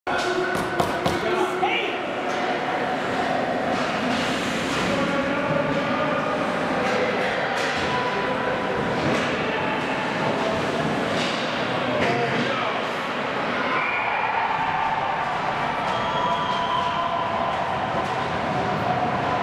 Troy 2 - USA Eagles vs Troy Sting - 09/20/19
Troy 2
USA Eagles vs Troy Sting - 09/20/19
Final Score: Troy - 4 , USA - 1